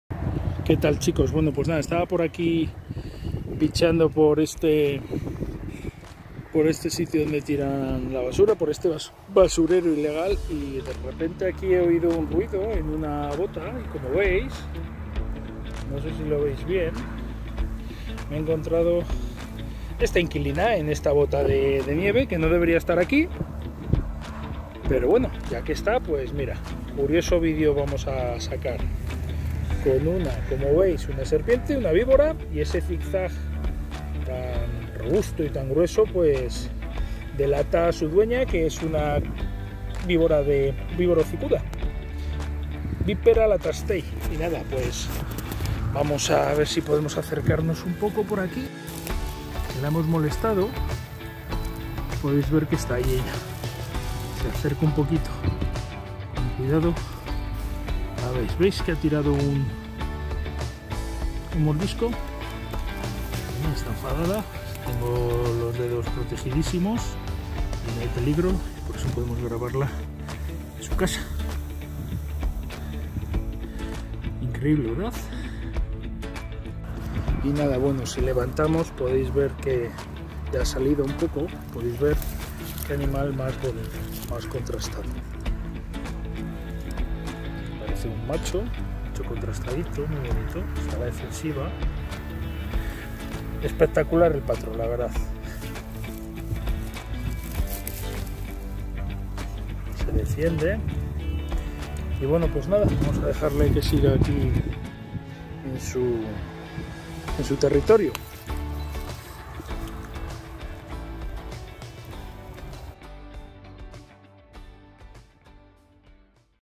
➡️ VÍBORA HOCICUDA (Vipera latastei) EN UNA BOTA DE ESQUÍ.
No son pocos los elementos donde hemos encontrado herpetos la verdad. Tales como váteres, zapatillas, cajones, armarios, bombonas, botellas etc.

te traemos un curioso vídeo de una Víbora hocicuda (Vipera latastei) que encontramos metida dentro de una bota de esquí, en un basurero. Una anécdota que nos sirve para enseñarte este bonito ejemplar.

Para más información sobre las víboras españolas, puedes visitar nuestro artículo relacionado